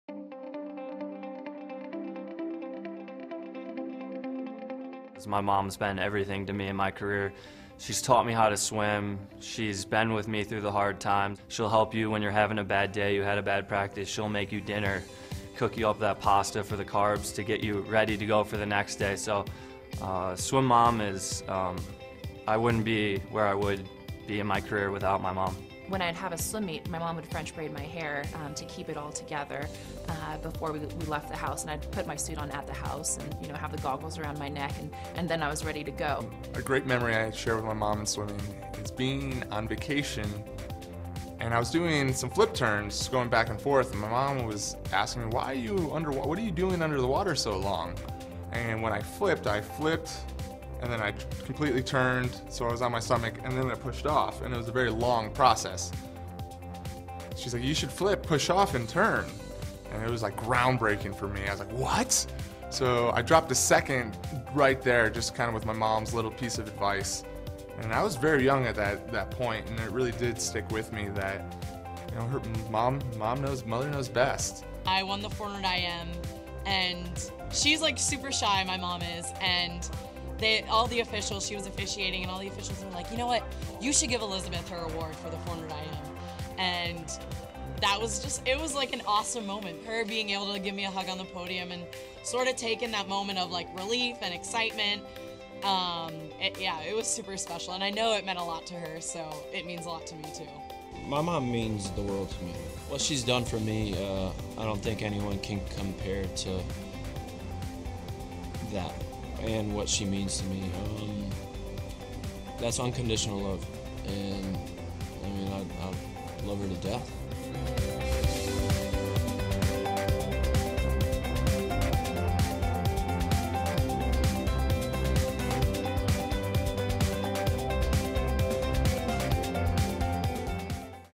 SwimToday: Thank You Swim Moms!
What makes swim moms so special? Five Olympic swimmers share why they love and appreciate their moms on Mother's Day! Want to learn tips and tricks for swim moms?